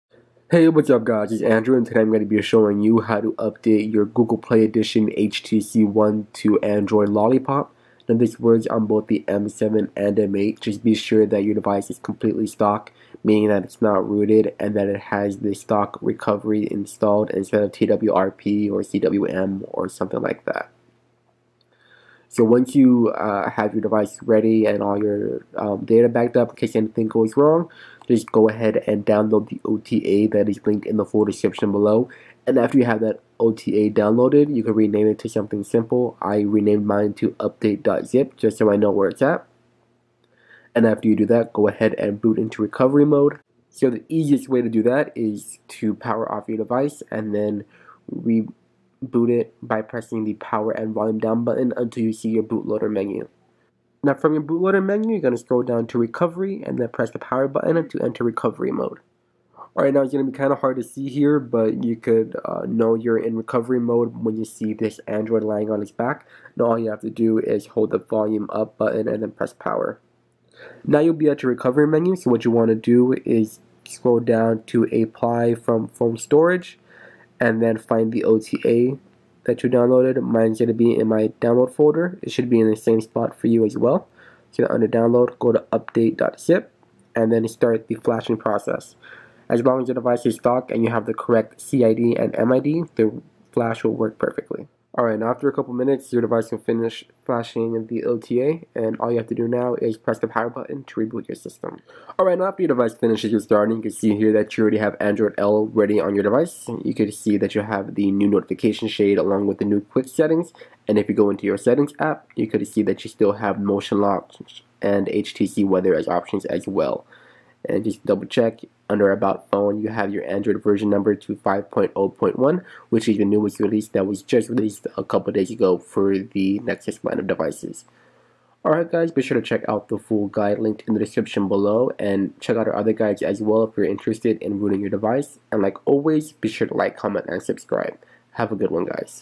Manually Update Your HTC One to Android 5.0 Lollipop [How-To]
Apply the Lollipop OTA Through Recovery [HTC One GPE]

In this tutorial, I'll show you how to use recovery mode to update your Google Play Edition HTC One M7/M8 to Lollipop. This is for those of you who have still not received their OTA notification and just cannot wait any longer.

The OTA can only be flashed through the stock recovery file, so you must have a completely stock device. You should also keep in mind this update will only work if you are coming from Android 4.4.4.

If you are using a Sense-based HTC One, check out the guide linked above for instructions on converting it to a GPE variant.